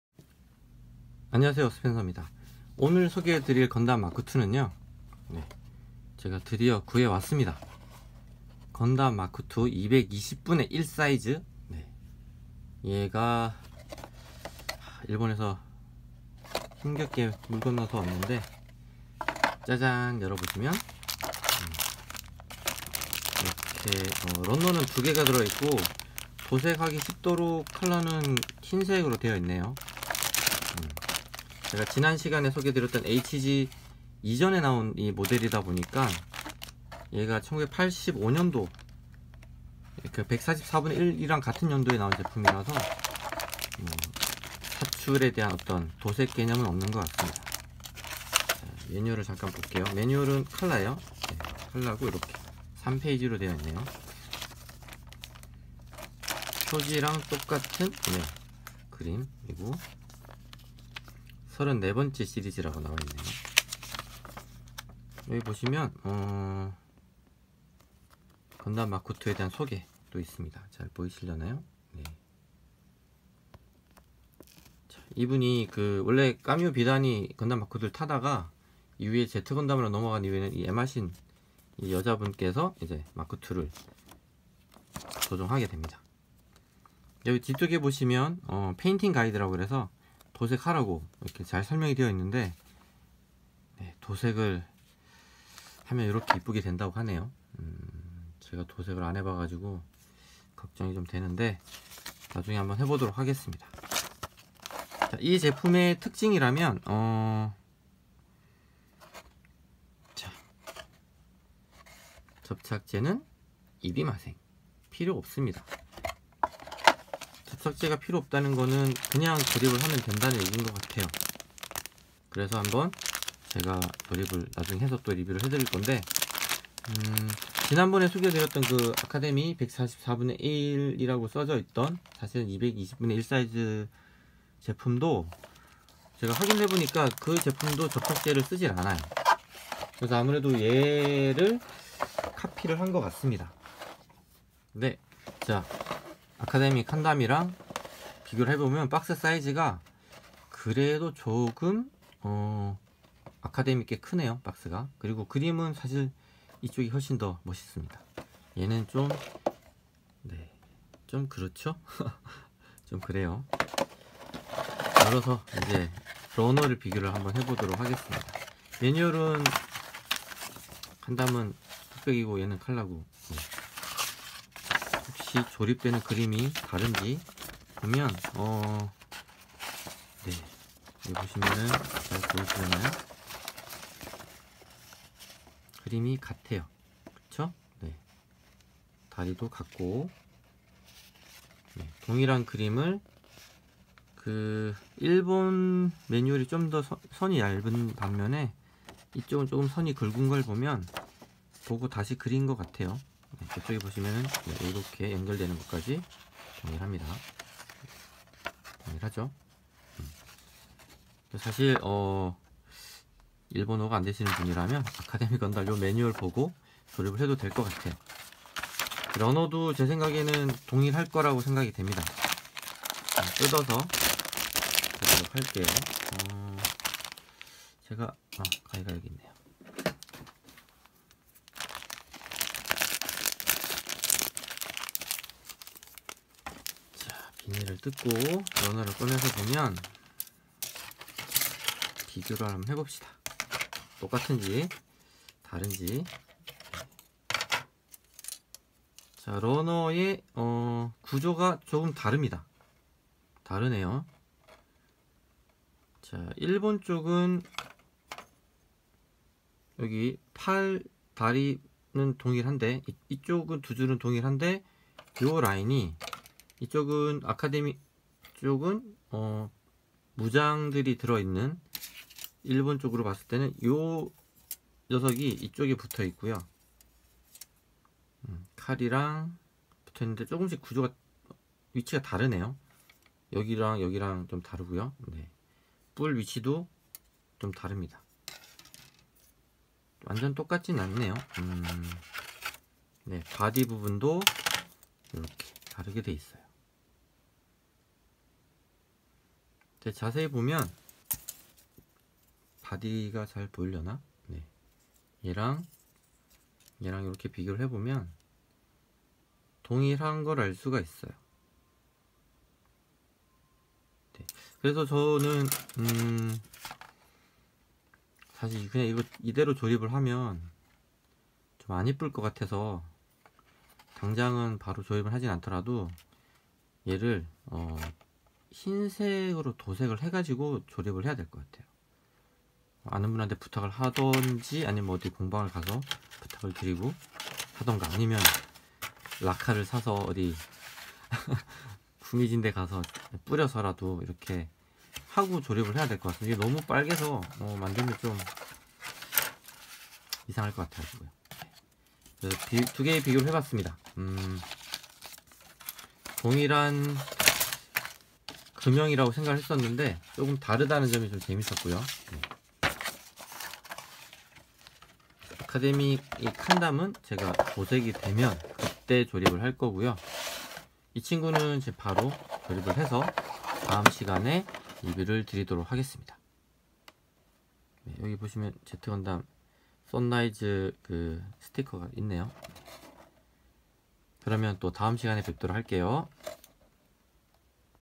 [건담마크2 소개 7편] 1:220 건담 마크 2 박스 리뷰편
1985년에 발매된 1:220 건담마크2 박스 리뷰 및 아카데미 칸담과 전격 비교!!